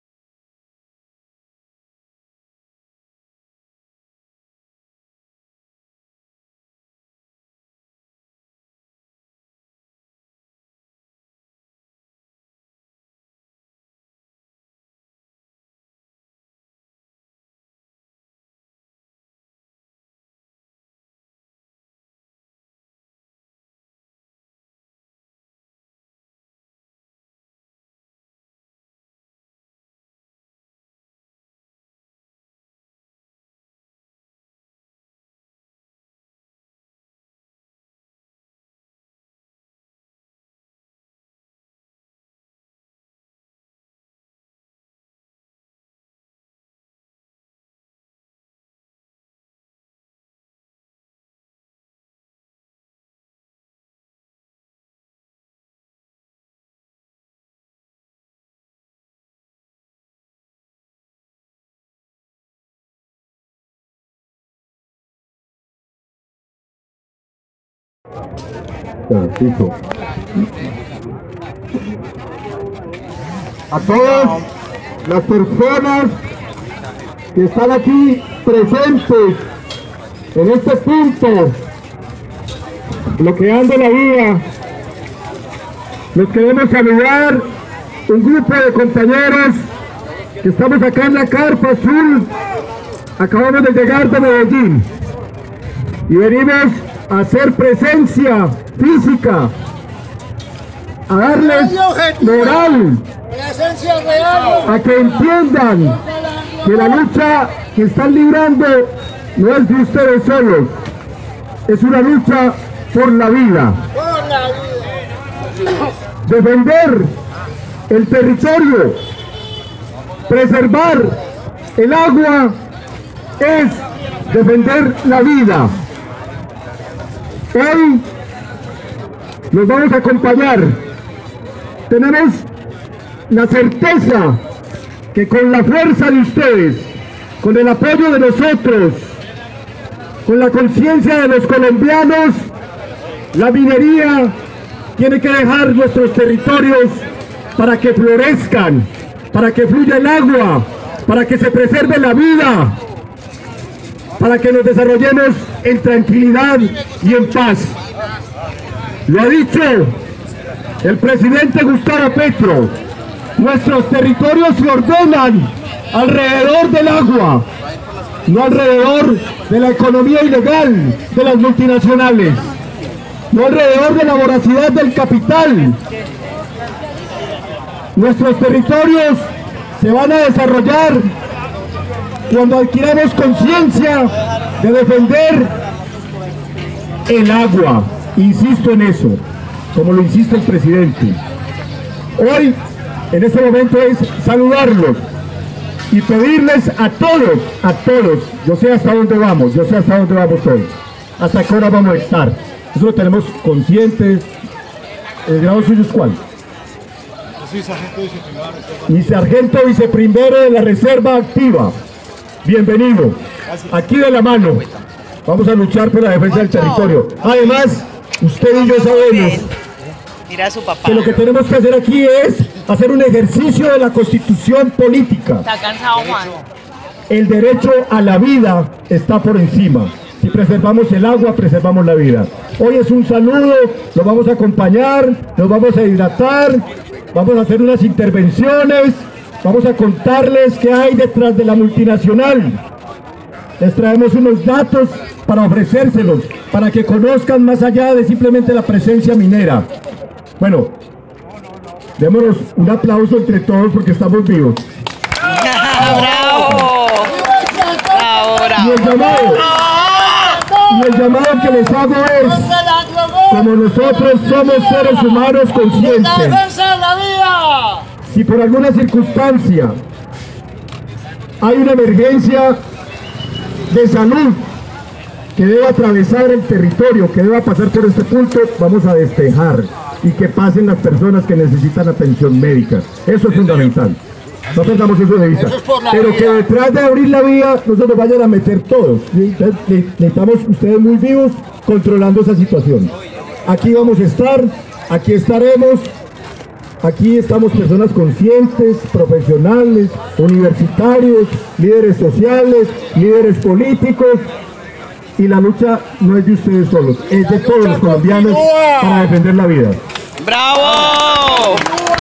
FUERA ANGLOGOLD DE JERICO ANTIOQUIA
Pese a protestas de campesinos y de la alcaldia contra la explotacion ilegal adelantada sin licencias ambientales y sin socializacion avanzan los trabajos para poner en operacion este projecto minero. Para detener esta catastrofe ambiental campesinos adelantaron una protesta en Supia, la cual fue apoyada por movimientos sociales y politicos desplazados al lugar desde Medellin. Habla, Francisco Garcia, de Colombia Humana, seccional Antioquia.